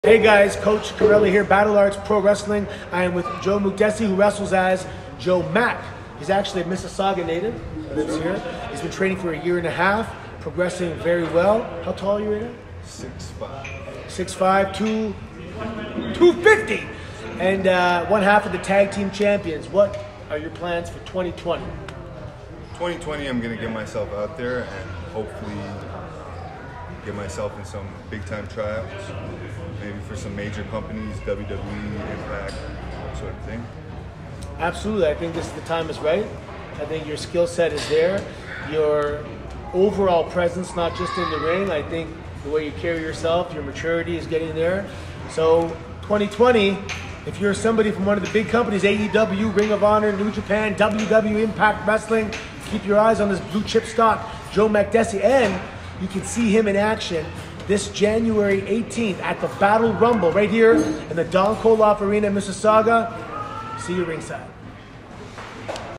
Coach Anthony Carelli (FKA Santino Marella) interview Joseph Moukdessi
See what Joe has in store for 2020 and prepare for the Battle Rumble